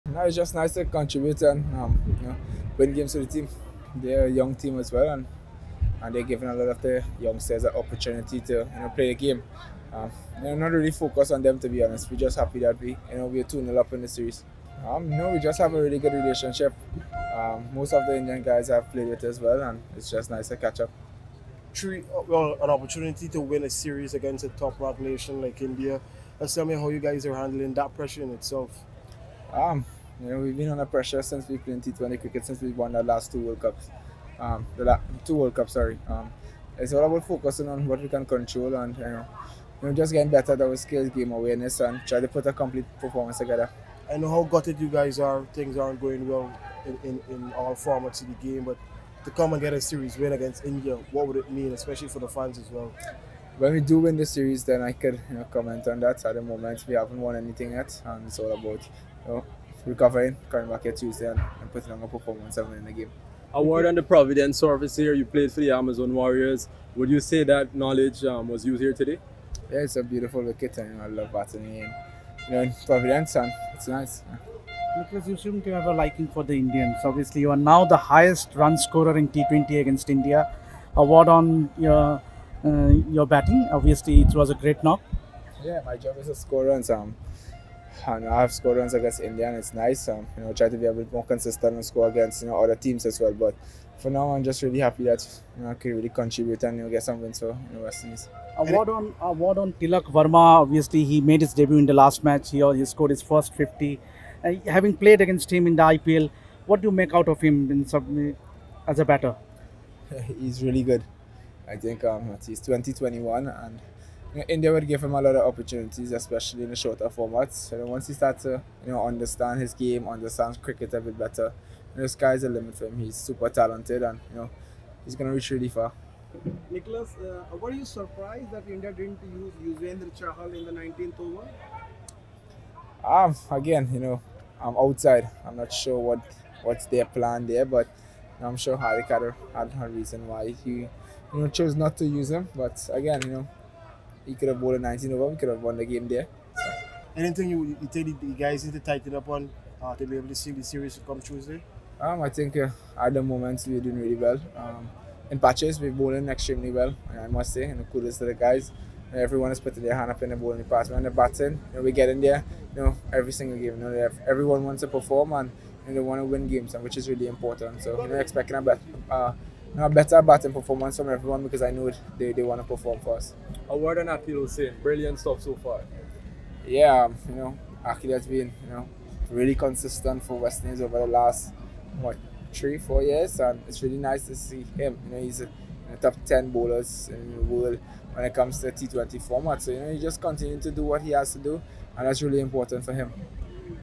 POORAN STARS AS WEST INDIES EXTEND SERIES LEAD
West Indies’ Nicholas Pooran was fined 15 per cent of his match fee for breaching the Level 1 ICC Code of Conduct in the second T20I against India. Pooran flouted Article 2.7 of the ICC Code of Conduct for Players and Player Support Personnel, which relates to public criticism in relation to an incident occurring in an international match.

The incident occurred following the review of an LBW decision in the fourth over of the India innings. Pooran criticised the umpires for having to use up a player review for a decision which he thought was clearly not out.

There was no need for a formal hearing after Pooran admitted to the offence and accepted the sanctions levelled by on-field umpires Leslie Reifer and Nigel Duguid, third umpire Gregory Brathwaite and fourth official Patrick Gustard and proposed by match referee Richie Richardson.

In addition, one demerit point has been added to Pooran’s disciplinary record, which was his first offence in a 24-month period.